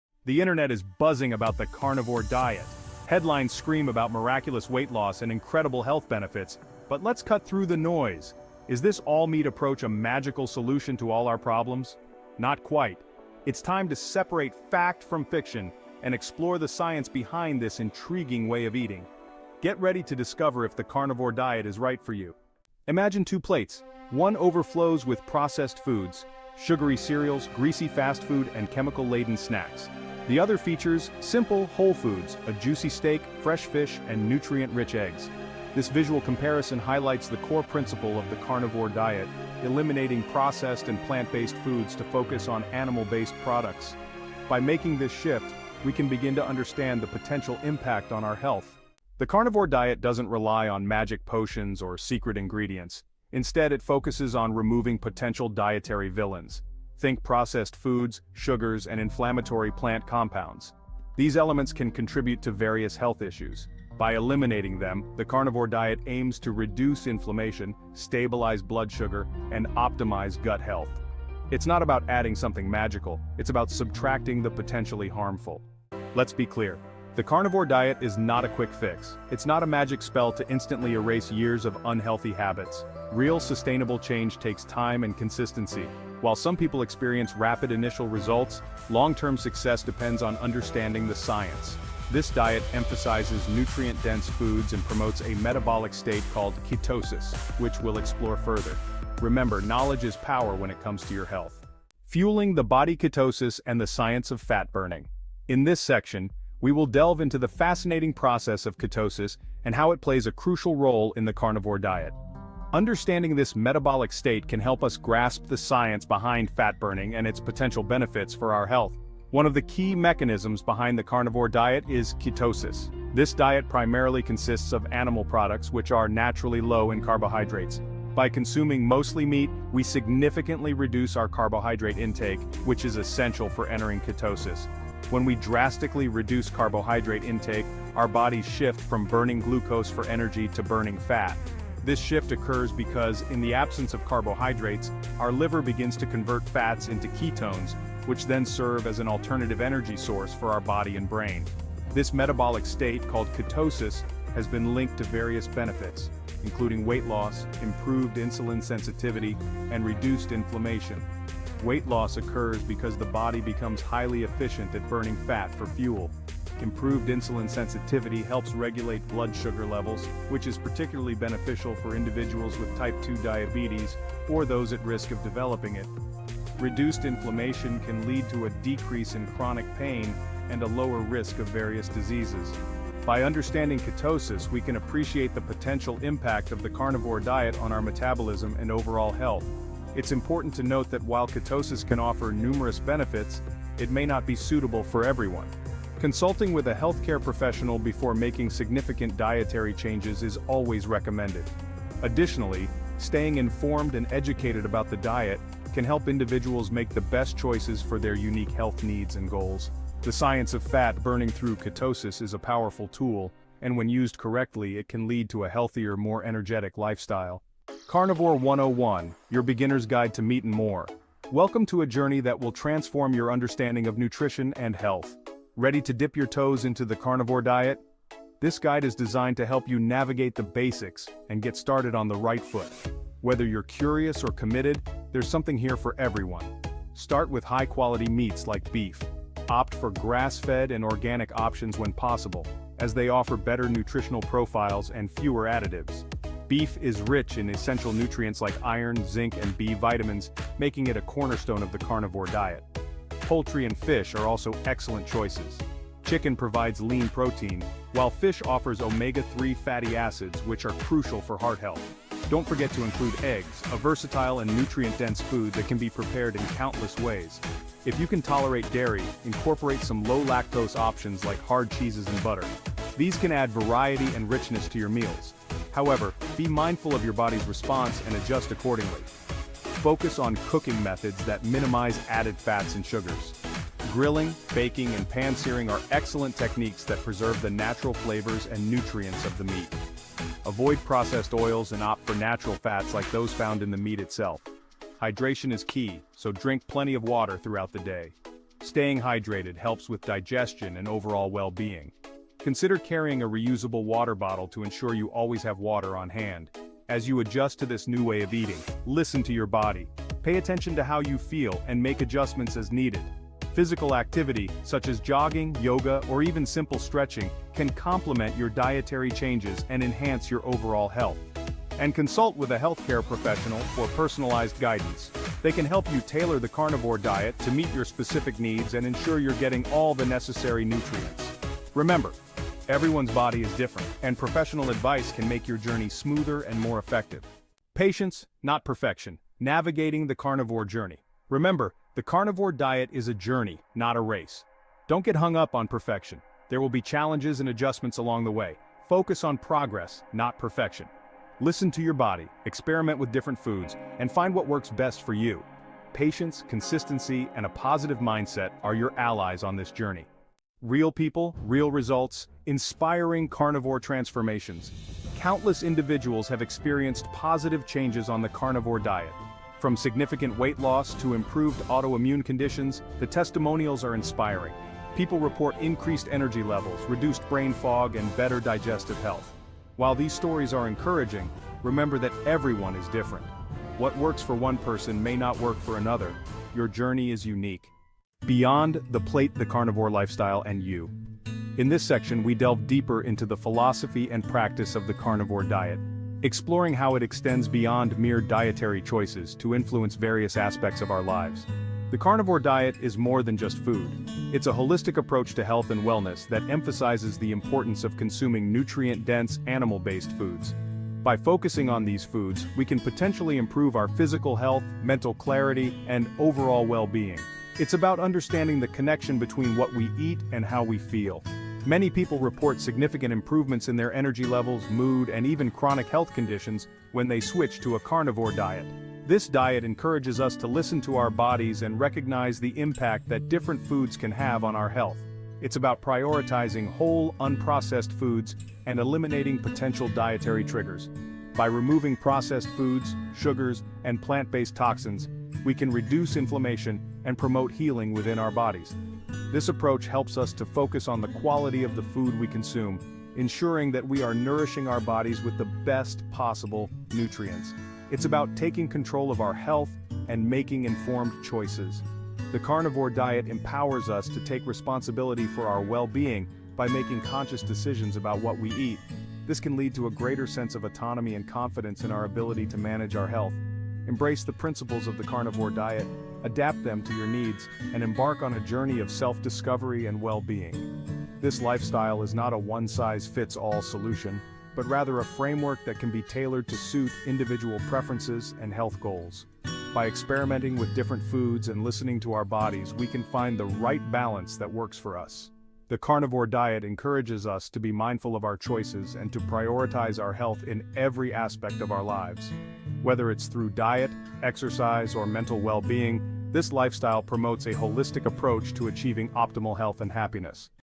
Carnivore Is Not A MAGIC Diet
In this video, we’re debunking the myth that the carnivore diet is a magic solution for health and weight loss. There’s nothing magical about it—it’s about what you remove from your diet, not what you add. By cutting out sugar, grains, and inflammatory foods, the carnivore diet allows your body to heal naturally and burn fat efficiently. We’ll dive deep into the carnivore diet benefits, real-life carnivore diet results, and how to get started with this powerful eating plan. Whether you’re just beginning or transitioning from keto to carnivore, this guide will walk you through the essentials, from meal plans to tips on achieving long-term success. Learn how the carnivore diet can be a game-changer for those looking to shed pounds and reduce inflammation. We’ll also touch on the connection between carnivore and keto, and how to combine the best of both worlds for optimal results. If you're curious about how to do the carnivore diet, this video is for you!

OUTLINE:
00:00:00 Miracle Cure or Marketing Hype?
00:00:22 Visualizing the Carnivore Difference
00:00:48 The Power of Elimination
00:01:15 Debunking the "Quick Fix" Myth
00:01:41 Ketosis and the Science of Fat Burning
00:03:37 Your Beginner's Guide to Meat and More
00:05:37 Navigating the Carnivore Journey
00:06:00 Inspiring Carnivore Transformations
00:06:27 The Carnivore Lifestyle and You